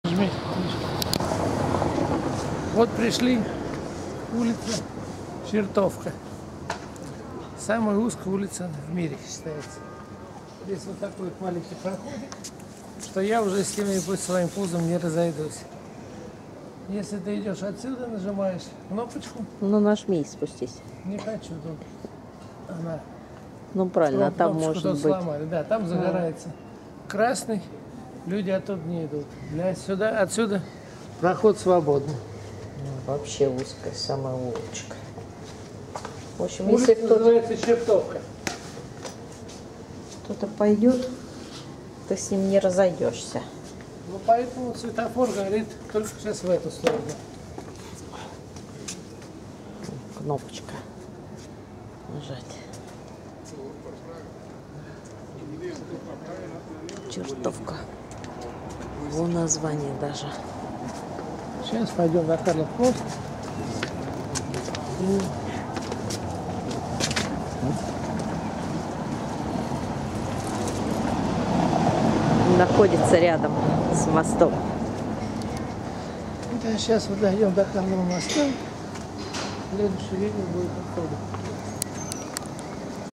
Чехия, Прага, отель Aladin, улица Чертовка, самая узкая улица в мире!
Пришли посмотреть улицу Чертовку!